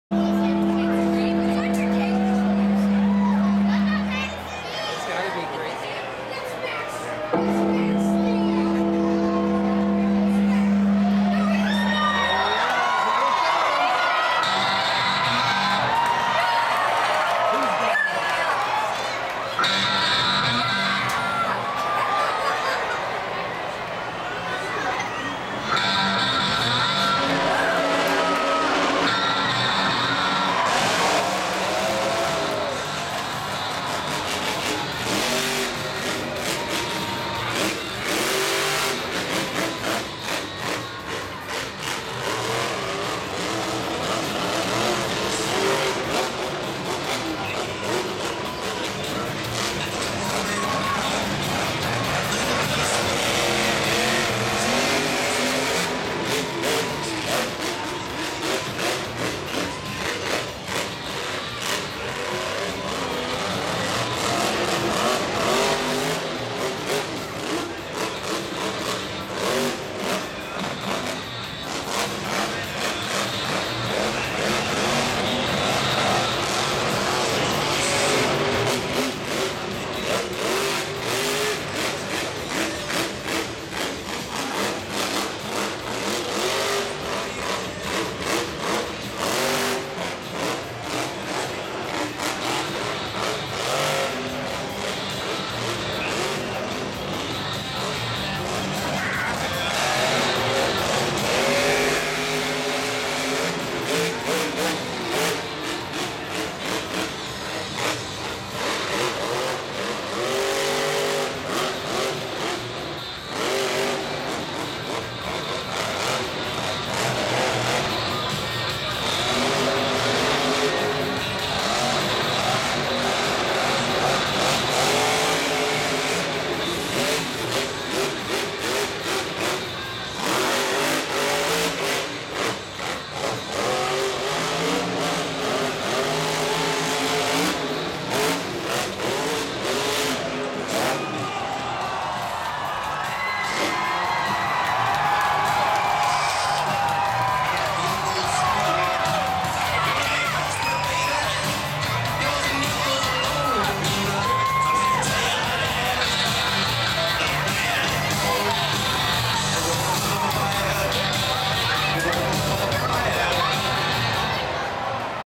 Grave Digger Freestyle M&T Stadium
Dennis Anderson doing what he does best. Max D stole the show though.